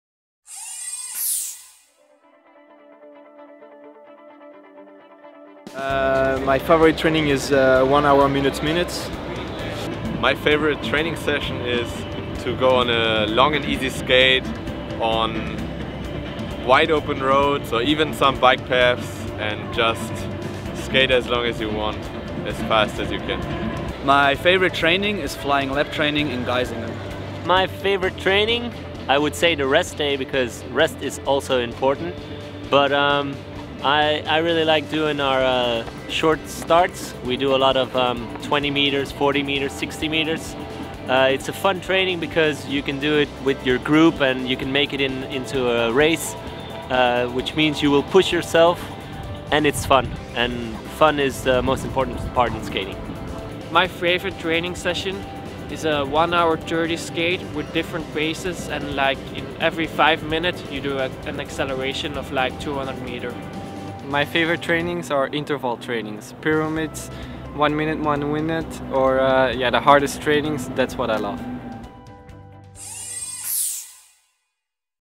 Powerskating Training 26: Pro training tips - Men
Powerskating Training Episode 26: Pro training tips - Men - Welcome to Powerskating! In episode 26, the Powerslide Matter Race Team members talk about their very own training tips.
In this series of Powerskating Training Episodes we will release video tutorials to improve your skating technique step by step.